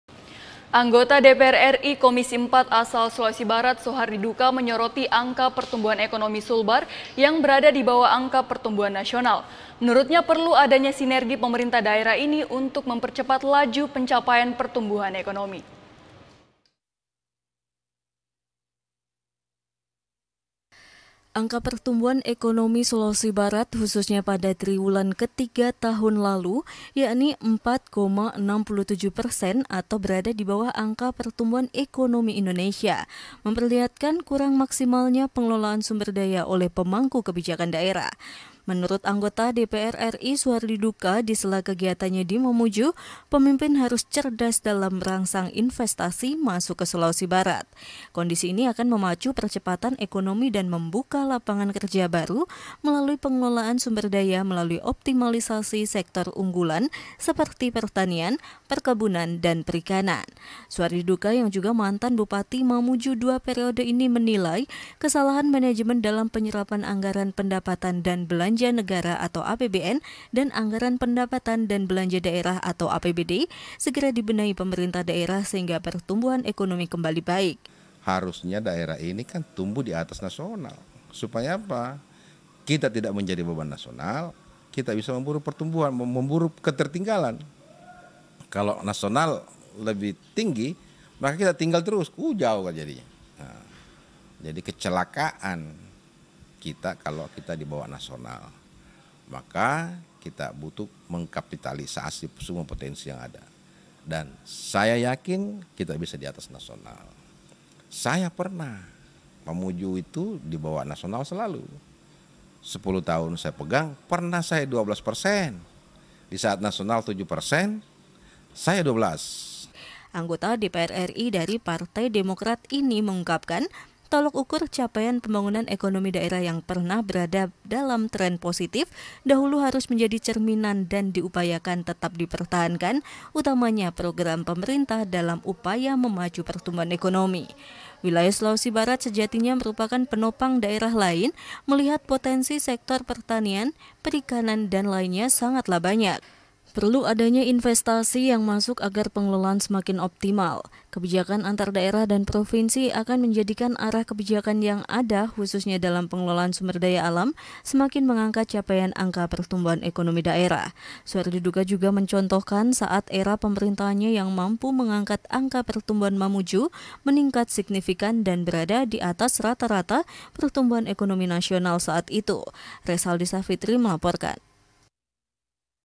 DPR-RI Soroti Ekonomi Sulbar Melemah | Sulawesi Barat Hari Ini | TVRI Sulbar, 5 Januari 2020
Anggota DPR-RI Komisi IV asal Sulawesi Barat Suhardi Duka, menyoroti angka pertumbuhan ekonomi Sulbar yang berada di bawah angka pertumbuhan nasional. Menurutnya perlu adanya sinergi pemerintah daerah ini mempercepat laju capaian pertumbuhan ekonomi.

Angka pertumbuhan ekonomi Sulawesi Barat khusunya pada Triwulan Ketiga tahun lalu yakni 4,67 persen atau berada di bawah angka pertumbuhan ekonomi indonesia memperlihatkan kurang maksimalnya pengelolaan sumber daya oleh pemangku kebijakan daerah. Menurut anggota DPR-RI Suhardi Duka di sela kegiatannya di Mamuju, pemimpin harus cerdas dalam merangsang investasi masuk ke sulbar. Kondisi ini akan memacu percepatan ekonomi dan membuka lapangan kerja baru melalui pengelolaan sumber daya melalui optimalisasi sektor unggulan seperti pertanian, perkebunan, dan perikanan.

Suhardi Duka yang juga mantan Bupati Mamuju dua periode ini menilai, kesalahan manajemen dalam penyerapan Anggaran Pendapatan Dan Belanja Negara – APBN dan Anggaran Pendapatan Dan Belanja Daerah – APBD segera dibenahi pemerintah daerah sehingga pertumbuhan ekonomi lebih baik.

Anggota DPR-RI dari Fraksi Partai Demokrat ini  mengungkapkan, tolok ukur capain pembangunan ekonomi daerah yang pernah berada dalam trend postif dahulu harus menjadi cerminan dan diupayakan tetap dipertahankan utamanya program pemerintah dalam upaya memacu pertumbuhan.Wilayah Sulawesi Barat sejatinya merupakan penopang daertah lain melihat potensi sektor pertanian, perikanan, dan lainnya sangatlah banyak. Perlu adanya investasi yang masuk agar pengelolaan semakin optimal. Padunya kebijakan antara daerah dan provinsi akan menjadikan arah kebijakan yang ada khusunya dalam pengelolaan Sumber Daya Alam semakin mengangkat capaian angka petumbuhan ekonomi daerah. Suhardi Duka juga mencontohkan saat era pemerintahannya yang mampu mengngkat angka pertumbuhan Kabupaten Mamuju meningkat signifikan dan berada di atas rata – rata pertumbuhan ekonomi nasional saat itu.
( Rezaldi Safitri - Melaporkan )

Reporter: REZALDI SAFITRI.
Kamerawan: WAHYU FEBRIAWAN.
Editor: SAMSUDDIN.
News Anchor: YOLANDA SIRAPPA.